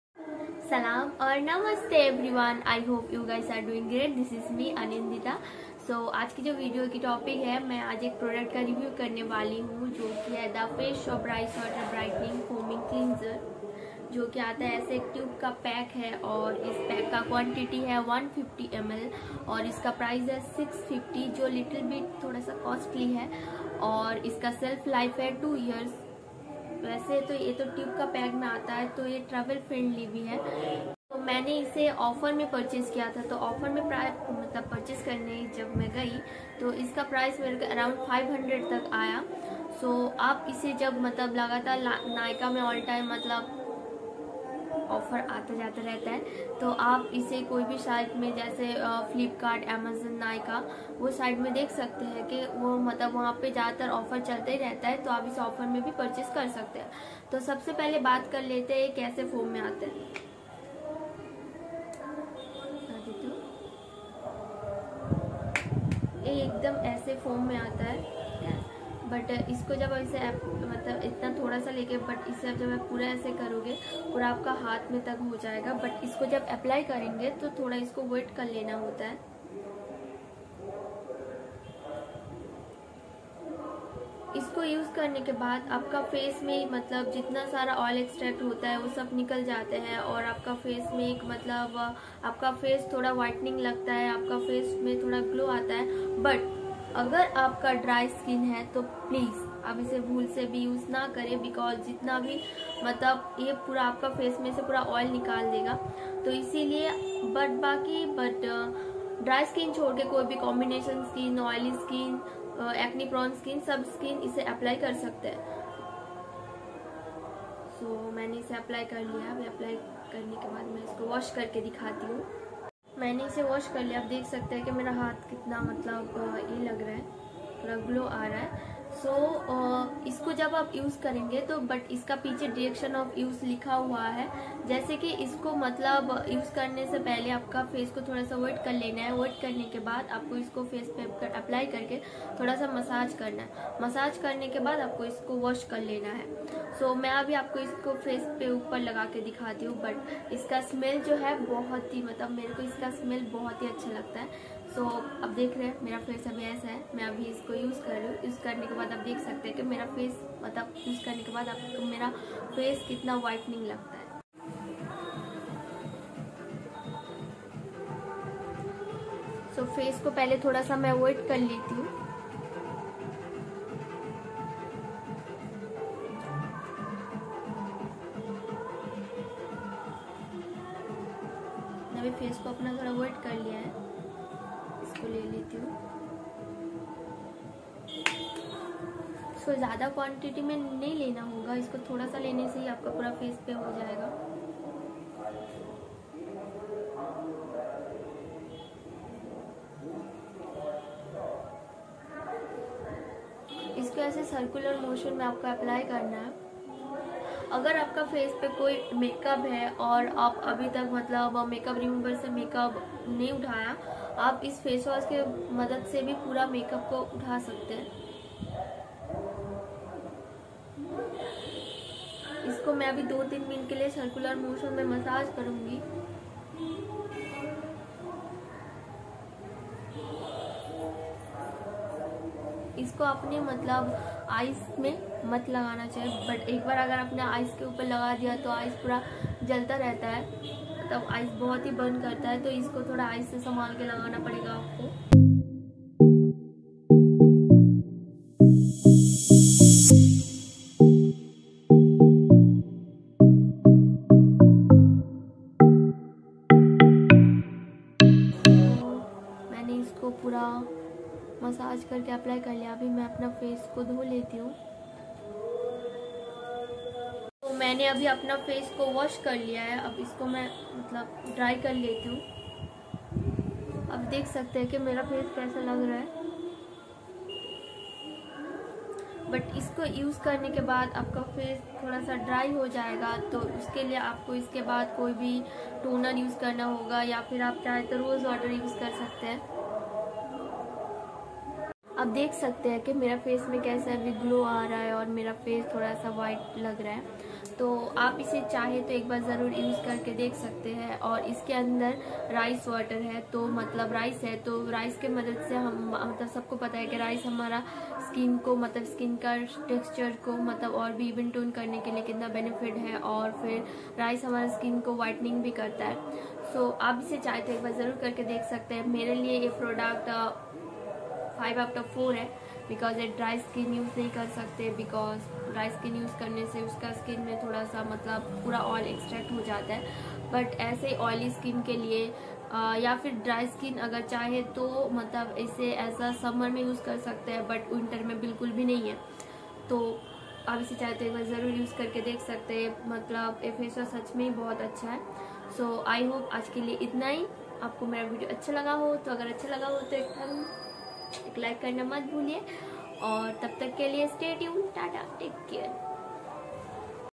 The FaceShop Rice Water Brightening Foaming Cleanser Review|Skin Brightening Face wash|SimpleZindegi
Hi everyone ❤️
In today's. video I review The Face Ship Rice Water Brightening Foaming Cleanser Usage, Demo, Benefits...

product details
The FaceShop Rice Water Brightening Foaming Cleanser

weight -150ml

Also, many fun and exciting videos on your way.